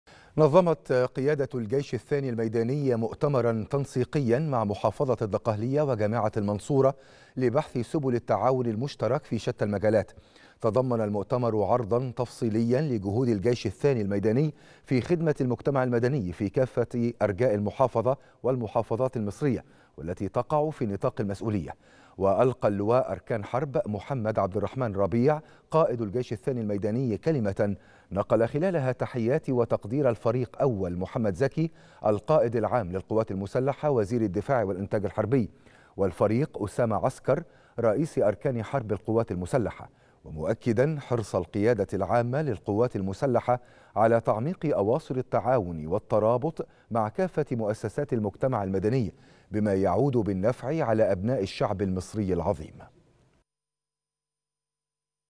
نشرة التاسعة | قيادة الجيش الثاني الميداني تنظم مؤتمرا تنسيقيا مع محافظة الدقهلية وجامعة المنصورة
نشرة التاسعة | قيادة الجيش الثاني الميداني تنظم مؤتمرا تنسيقيا مع محافظة الدقهلية وجامعة المنصورة لبحث أوجه التعاون المشترك لخدمة المجتمع المدني